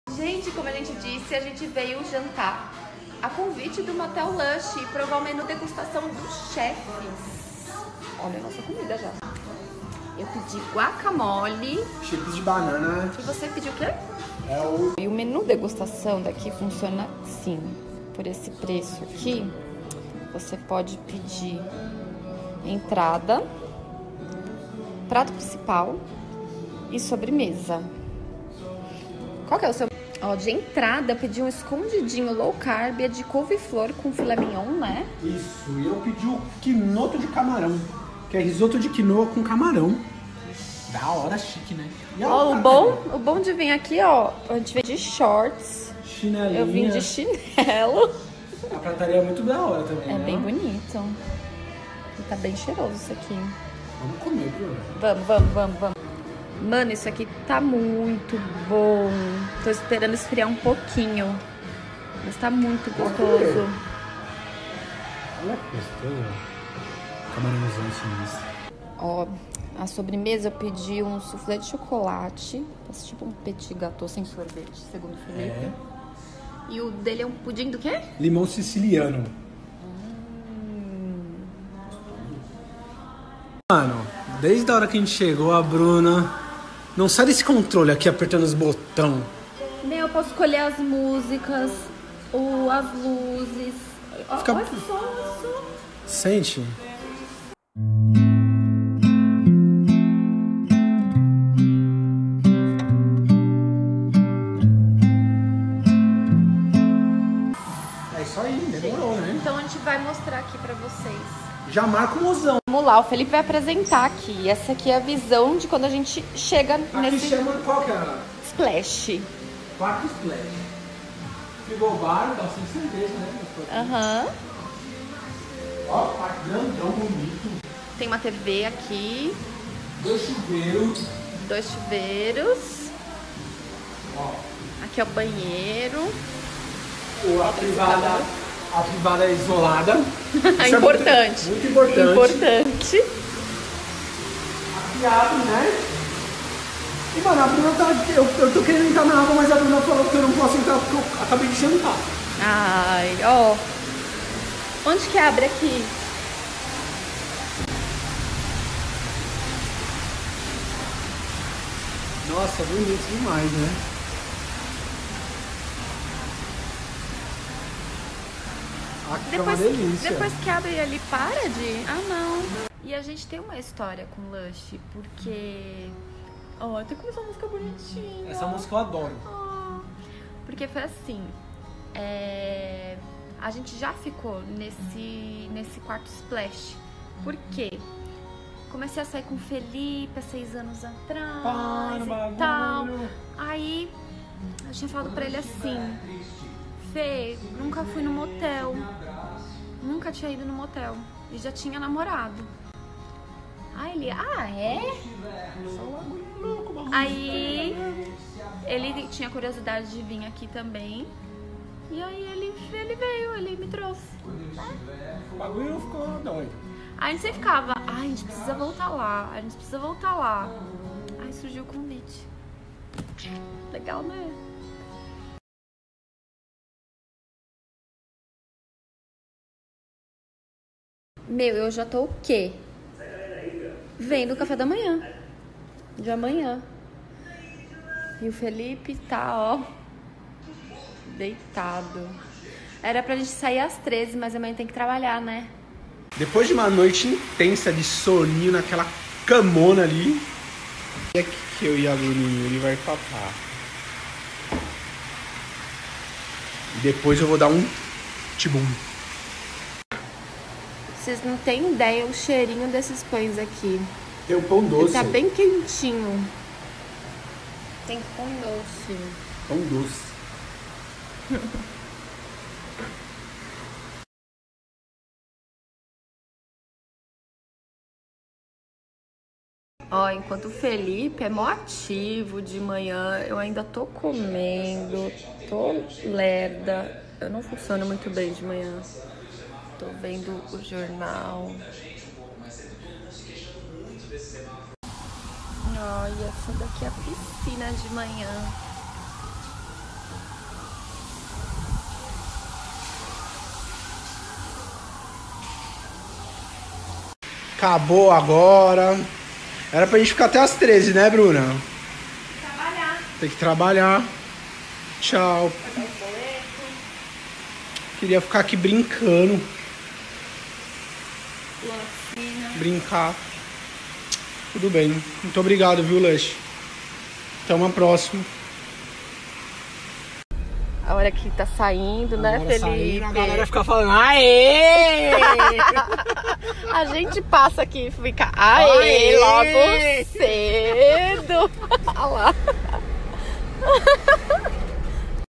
[ExperimenteEmSP] - Motel Lush
Como eles mesmos falam, o Lush é um “resort urbano privado”. O quarto Splash que ficamos (inclusive é o mesmo que conhecemos em 2013) tem teto solar, piscina climatizada, dois chuveiros, televisão, rádio própria do motel com vários estilos musicais, tem quarto que tem cinema 4D, ou seja, dá vontade de morar lá. Hahaha⠀⠀⠀⠀
Motel Lush
📍Av. do Estado, 6600 - Cambuci
Menu degustação: R$ 99